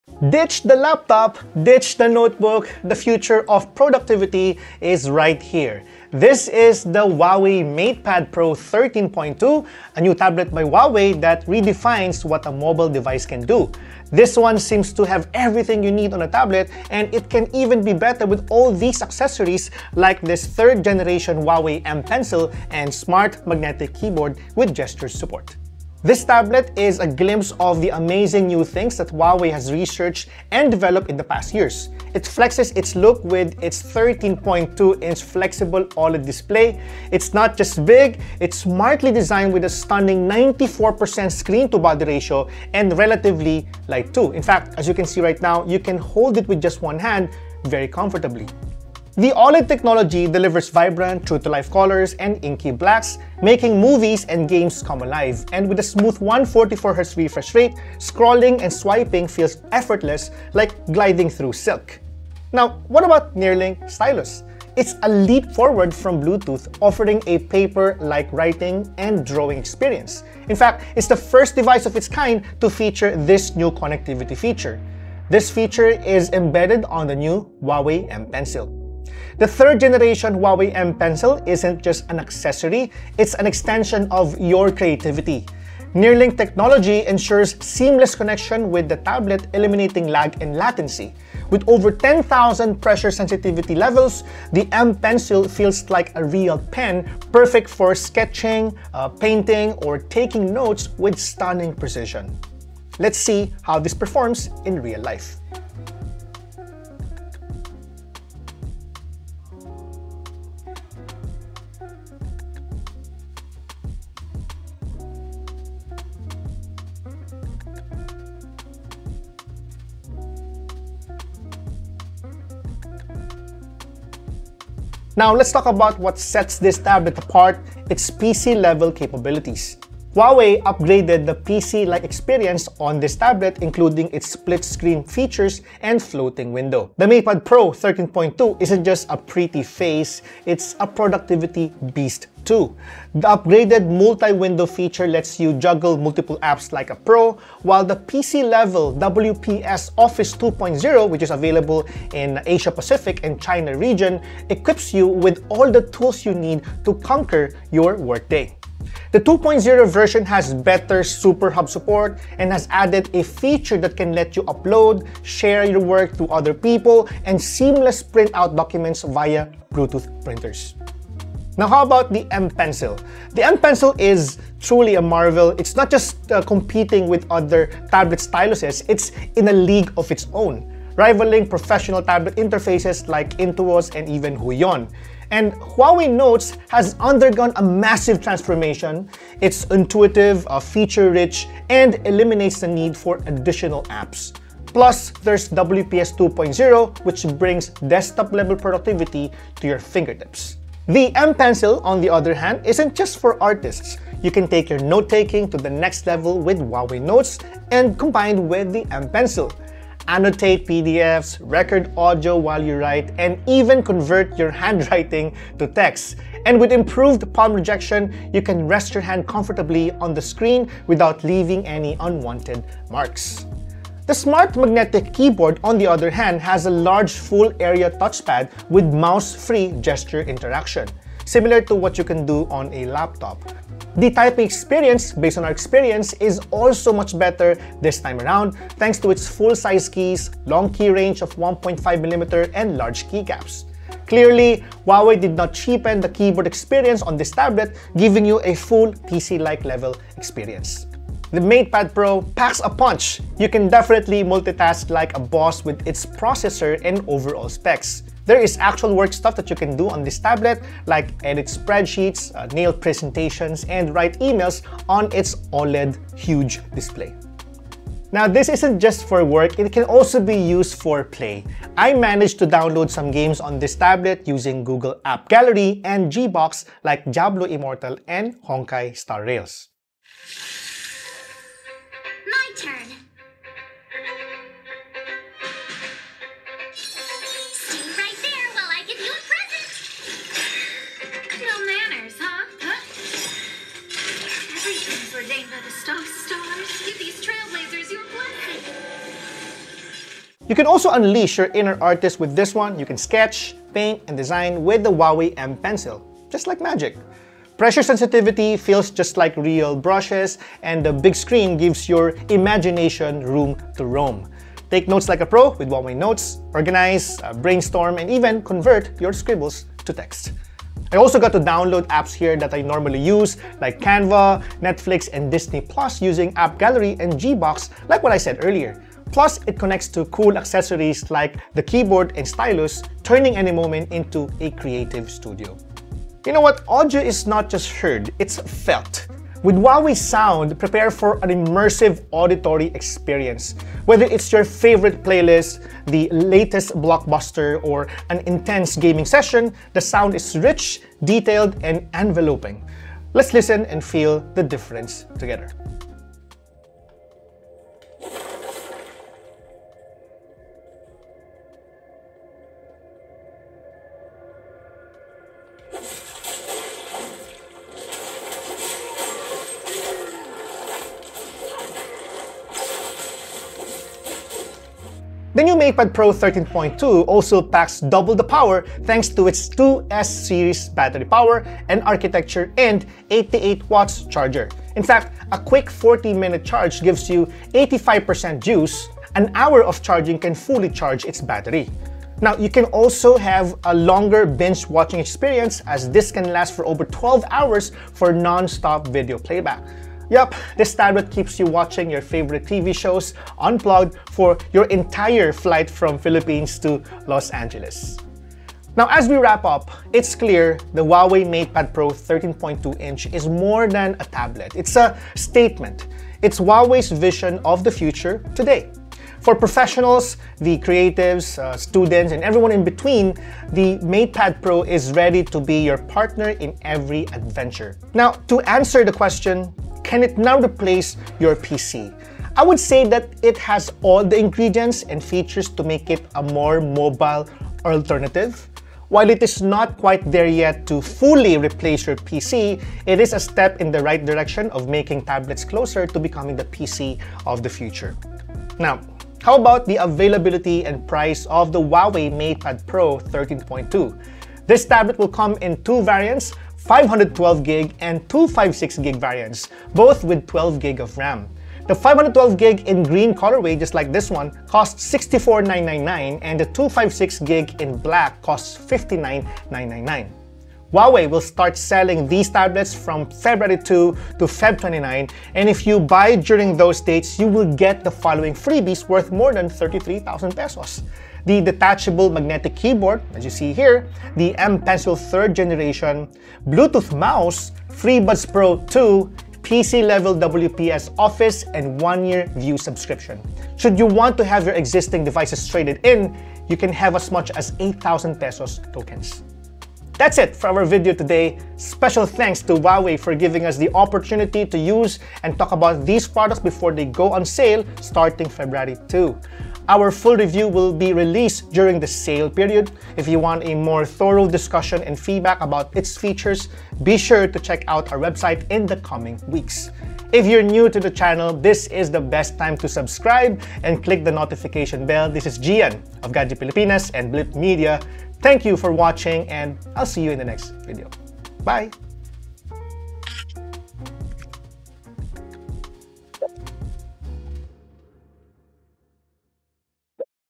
Huawei MatePad Pro 13.2": Can It Replace Your PC?
Tablets don't usually come in a package that can outperform modern laptops. The Huawei MatePad Pro 13.2 seems to break the standards to ditch the thoughts of getting a bulkier laptop.

The Huawei MatePad Pro 13.2 is now official for as low as PhP59,999.

We were also informed by Huawei that the PC-Level WPS 2.0 that we used in this video will be available on AppGallery on February 6!

Line, bar and pie charts by Flourish team